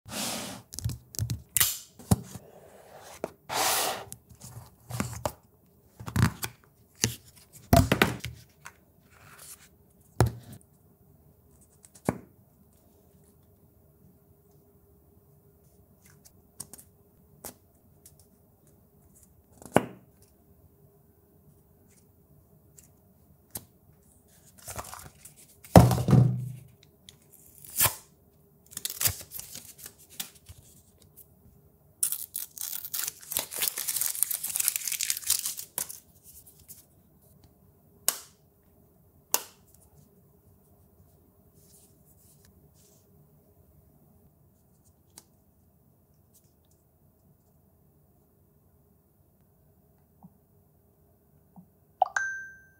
Google Pixel Buds Pro Unboxing! ✨ASMR✨
The Pixel Buds Pro are here and this is what it comes with in the box!

The Unboxing Knife I used
Google Pixel Buds

The camera I used to film this
Microphone I used
Other Microphone I used

If you see this, comment FodaAdu​​​